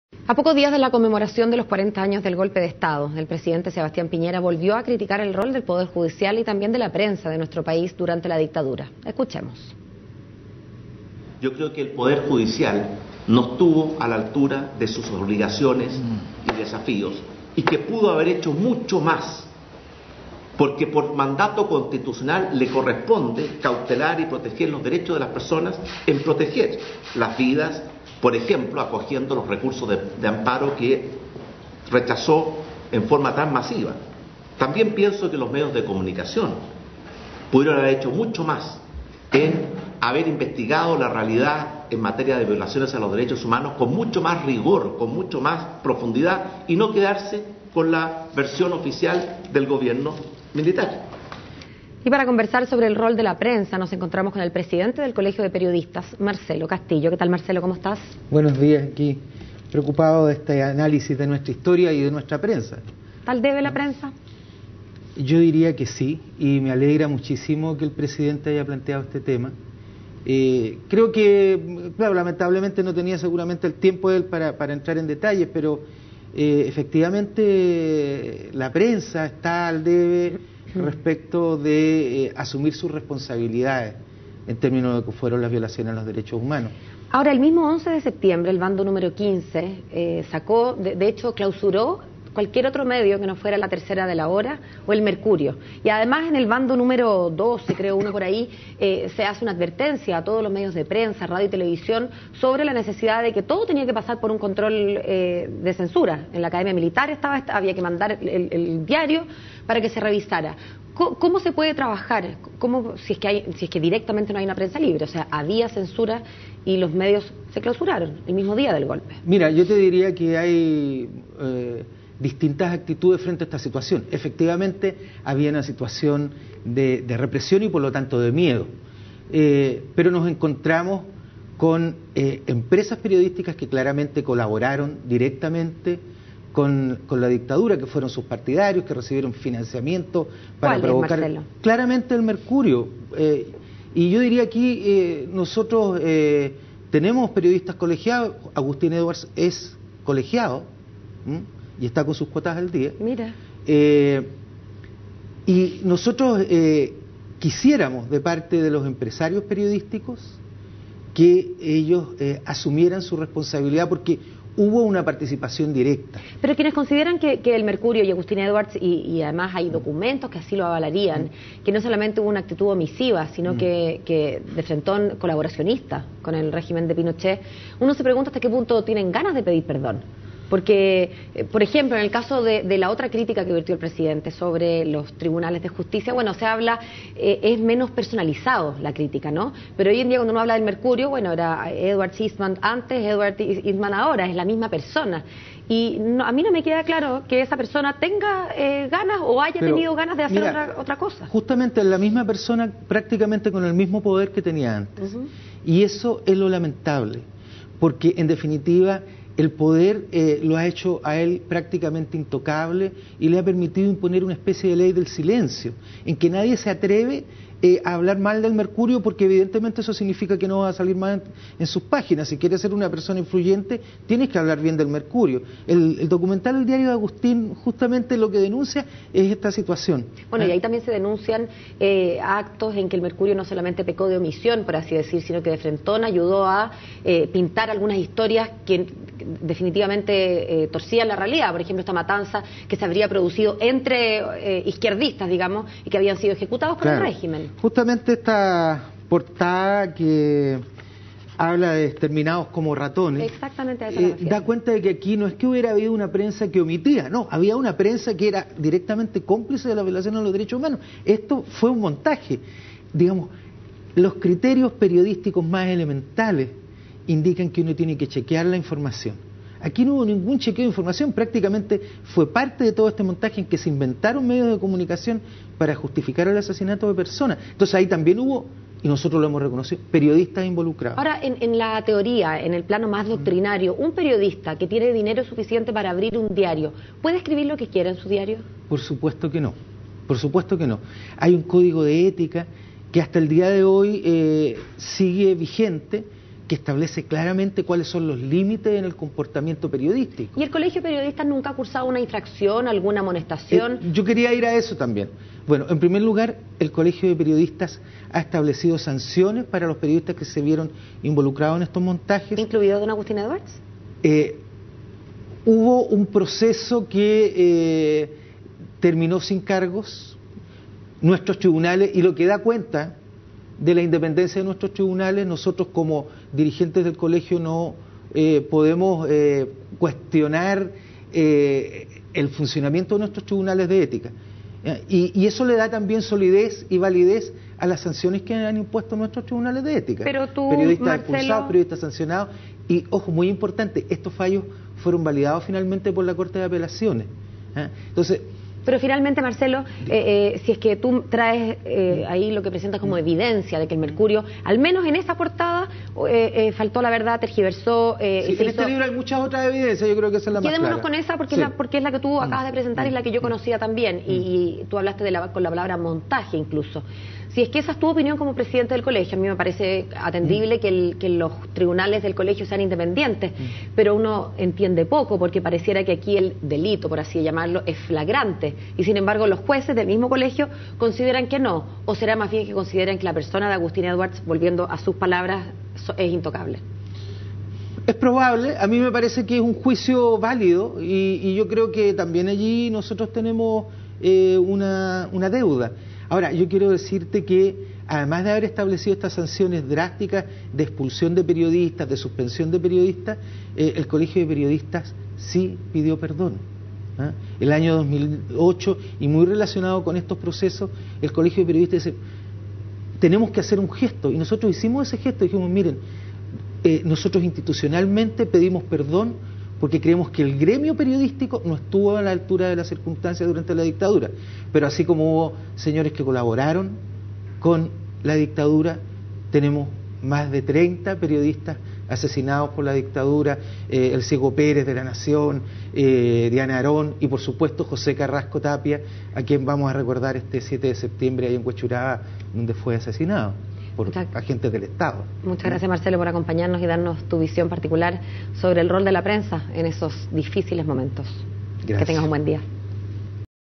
Colegio de Periodistas: "La prensa debe asumir su responsabilidad en las violaciones a los D.D.H.H"
Marcelo Castillo, se refirió a las críticas del presidente Sebastián Piñera, con respecto al rol de la prensa durante la dictadura.